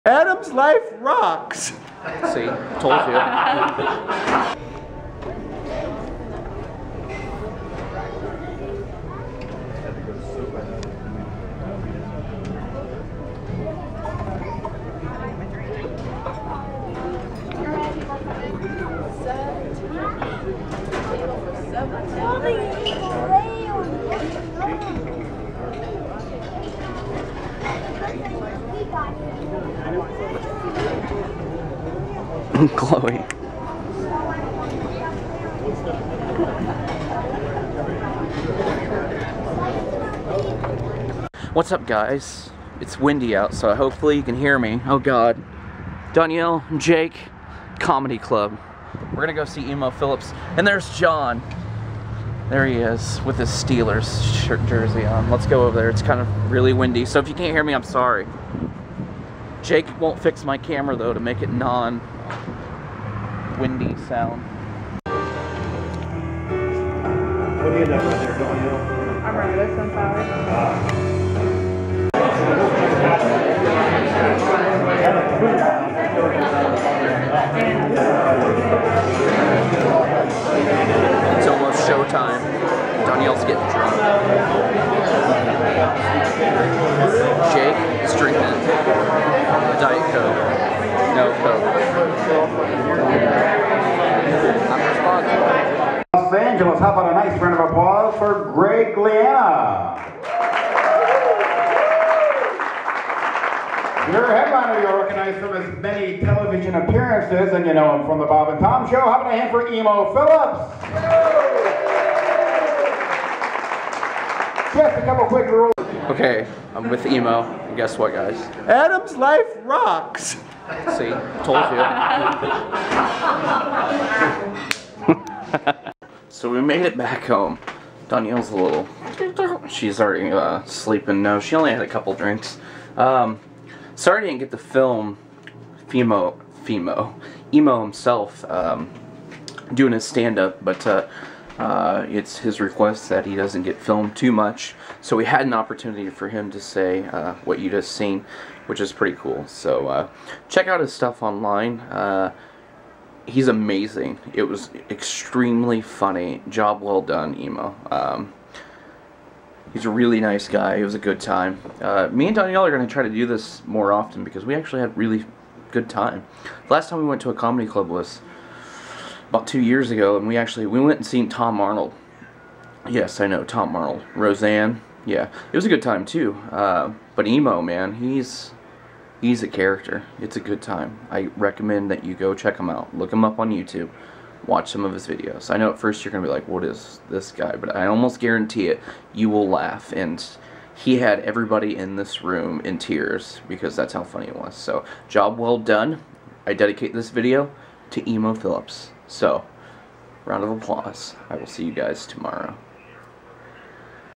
Oh That Emo Philips..
Dawnyelle and Adam hit the club for a night of laughs in Mason City, IL. Emo Philips is doing a show and we get to meet him after the show.

▼The Basement ▼

~MAIL~

Adam W
Decatur, IL 62524 ●Adam's Life Fun Stuff●

●Ghost Stuff●

●Photos,films,blogs●